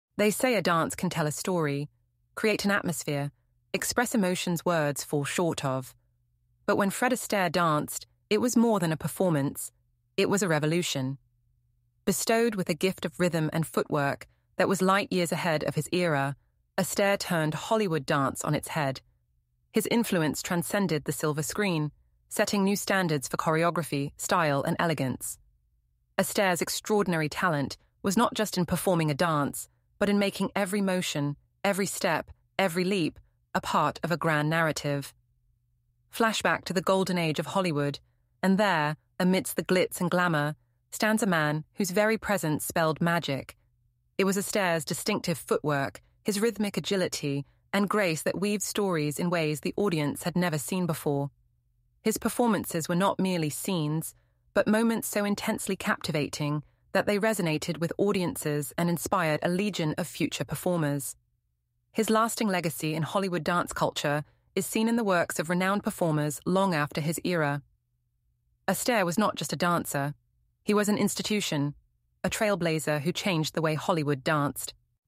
🎬 Fred Astaire | The King of Tap Dance!
🎬✨ Hey there, movie buffs and dance aficionados! 💃🕺 Do you know the man who transformed Hollywood with his dazzling dance moves and timeless charisma? That's right, it's none other than the legendary Fred Astaire! 🌟

From tap-dancing his way into our hearts to setting the gold standard for musical films, Fred Astaire's impact on Tinseltown is nothing short of iconic. 🎩🎥✨

🔍 Want to dive into the glitz and glam of his storied career? Curious about the secrets behind his seamless routines and groundbreaking performances? 🤔

📺 WATCH NOW to discover why Fred Astaire is still the king of dance and a true Hollywood legend! 👑🌠

00:00:00 they say a dance can tell a story create an atmosphere express emotions words fall short of but when Fred a stair danced it was more than a performance it was a revolution bestowed with a gift of Rhythm and footwork that was light years ahead of his era a stare turned Hollywood dance on its head his influence transcended the Silver Screen setting new standards for choreography style and elegance A's extraordinary Talent was not just in performing a dance but but in making every motion every step every Leap a part of a grand

00:00:35 narrative flashback to the Golden Age of Hollywood and there amidst The Glitz and glamour stands a man whose very presence spelled magic it was a stairs distinctive footwork his rhythmic agility and Grace that weaved stories in ways the audience had never seen before his performances were not merely scenes but moments so intensely captivating that they resonated with audiences and inspired a legion of future performers his lasting Legacy in Hollywood dance culture is seen in the works of renowned

00:01:06 performers long after his era aair was not just a dancer he was an institution a Trailblazer who changed the way Hollywood danced